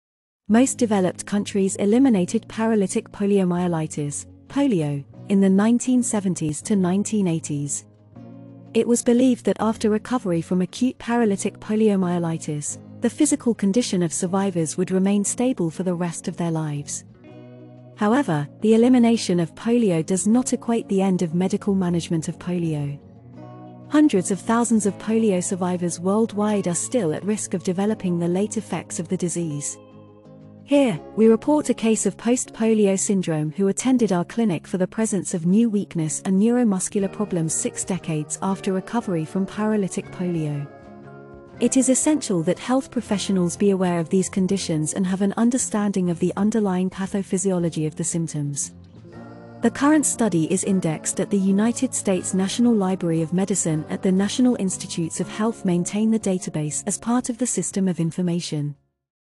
Post-poliomyelitis syndrome
Most developed countries eliminated paralytic poliomyelitis (polio) in the 1970s to 1980s.

It was believed that after recovery from acute paralytic poliomyelitis, the physical condition of survivors would remain stable for the rest of their lives.

However, the elimination of polio does not equate the end of medical management of polio.

Hundreds of thousands of polio survivors worldwide are still at risk of developing the late effects of the disease.

Here, we report a case of post-polio syndrome who attended our clinic for the presence of new weakness and neuromuscular problems six decades after recovery from paralytic polio.
It is essential that health professionals be aware of these conditions and have an understanding of the underlying pathophysiology of the symptoms.

Chu, Eric Chun Pu, and Kary Ka Wai Lam. “Post-poliomyelitis syndrome.” International medical case reports journal vol. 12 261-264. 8 Aug. 2019, doi:10.2147/IMCRJ.S219481